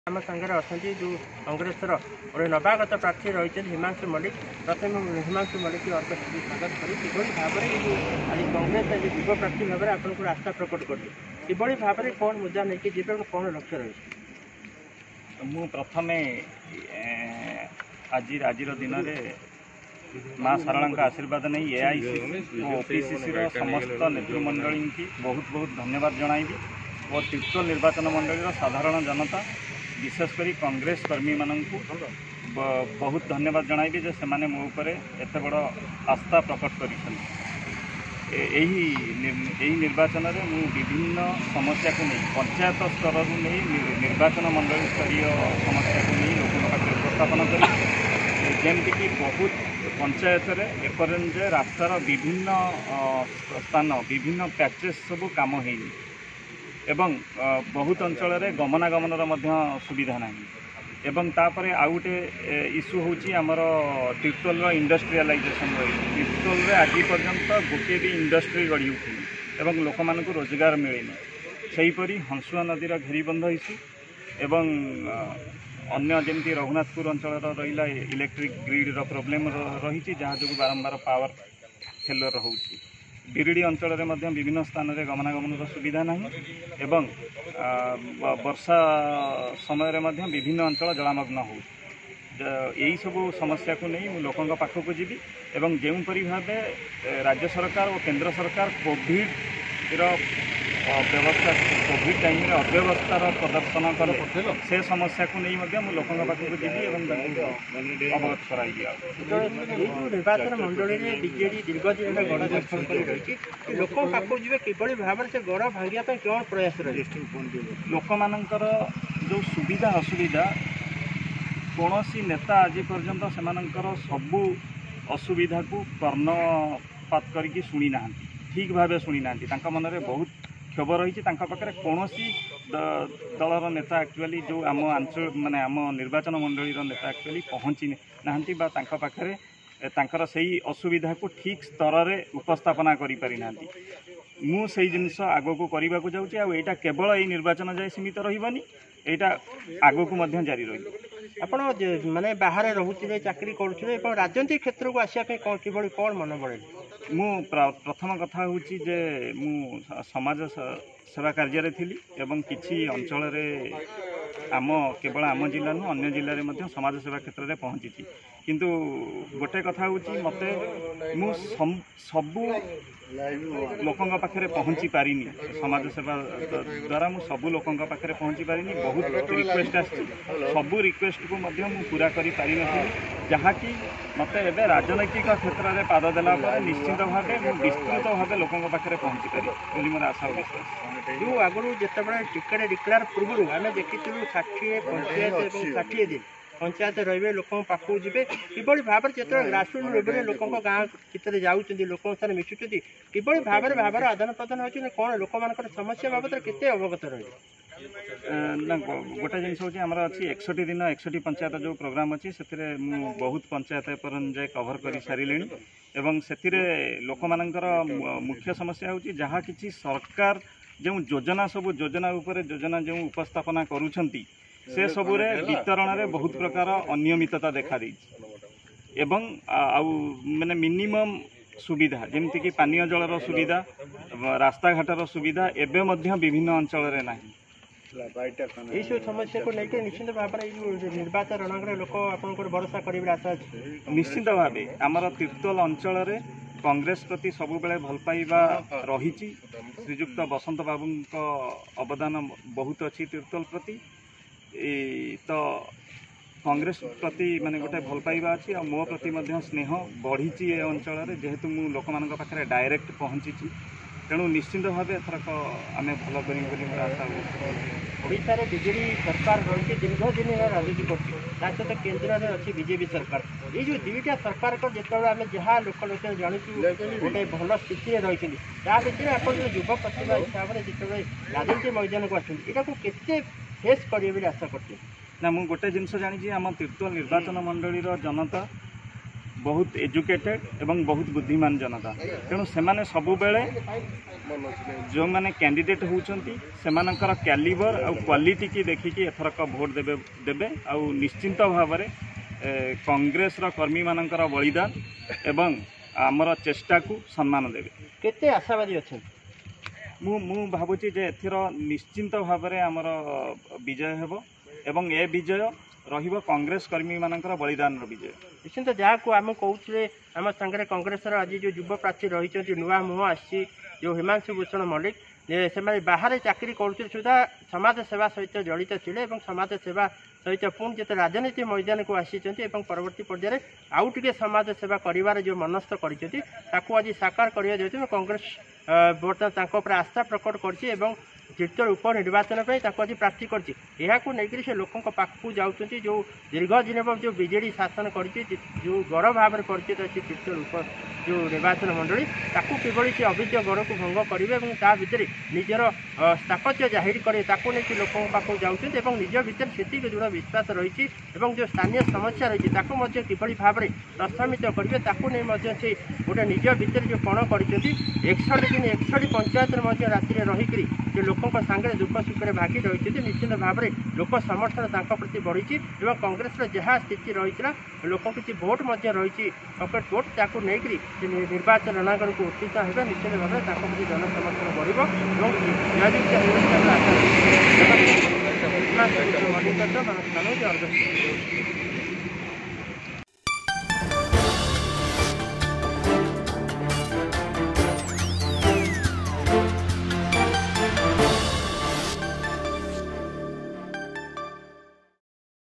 ଟିକେଟ ପାଇବା ପରେ ଶୁଣନ୍ତୁ କଣ କହୁଛନ୍ତି ହିମାଂଶୁ ଭୂଷଣ ମଲ୍ଲିକ
ଜଗତସିଂହପୁର ଜିଲ୍ଲା ତିର୍ତ୍ତୋଲ ନିର୍ବାଚନ ମଣ୍ଡଳୀ ଉପ ନିର୍ବାଚନ ପାଇଁ କଂଗ୍ରେସ ପ୍ରାର୍ଥୀ ଘୋଷଣା କରିଛି କଂଗ୍ରେସ ପ୍ରାର୍ଥୀ ଭାବେ ନବାଗତ ହିମାଂଶୁ ଭୂଷଣ ମଲ୍ଲିକଙ୍କୁ ଦଳ ପ୍ରାର୍ଥୀ କରିଛି ଟିକେଟ ପାଇବା ପରେ ଶୁଣନ୍ତୁ ଏନେଇ କଣ କହୁଛନ୍ତି.